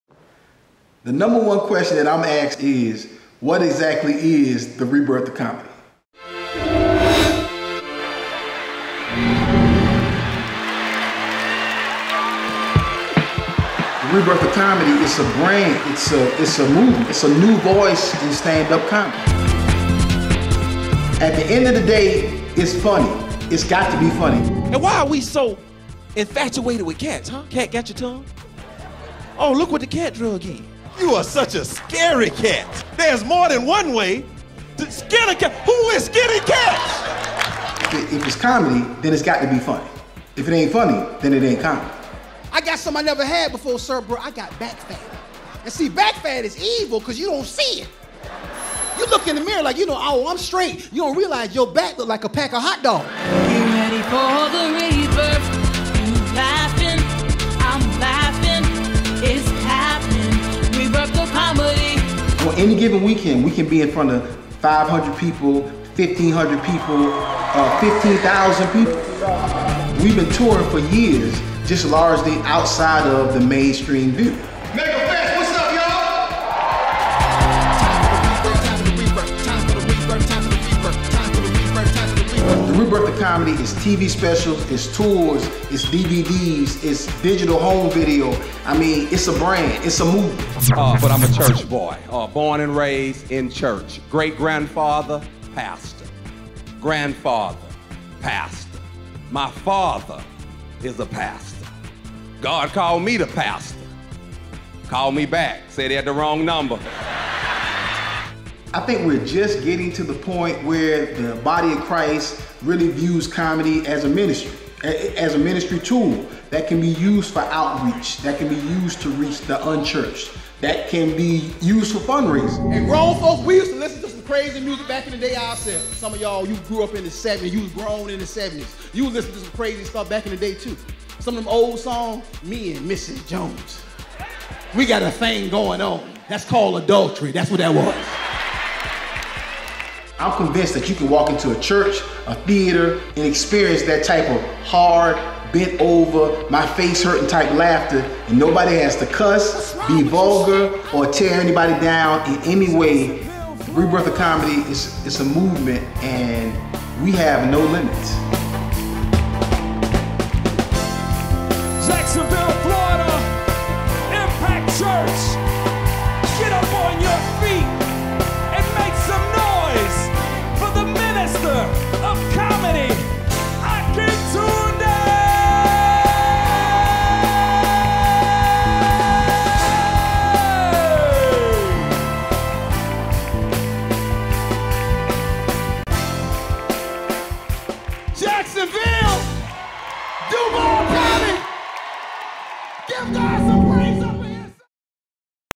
WHAT IS THE REBIRTH OF COMEDY - SIZZLE REEL
Comedian Akintunde sits down for an interview to answer the frequently asked question, "What is the Rebirth of Comedy?"